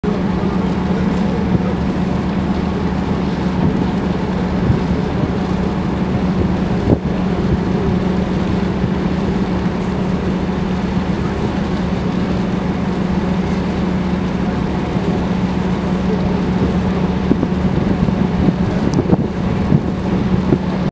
C.T.M.A. MADELEINE
Arriving in Cap-aux-Meules, Iles-de-la-Madeleine on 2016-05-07.